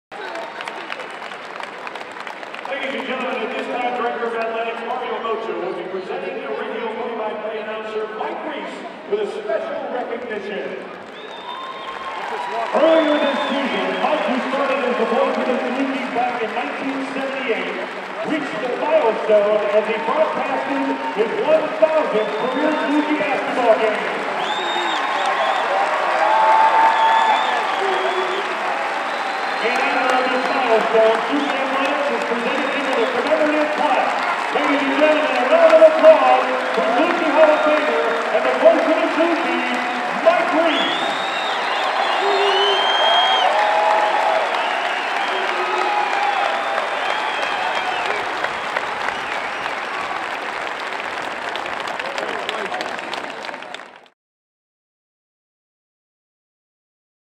Mike Reis Honored with Special Recognition
Last year, Saluki Athletics honored broadcast veteran Mike Reis at halftime of the SIU vs. UNI men's basketball game for broadcasting over 1,000 Saluki games over his 34 year career.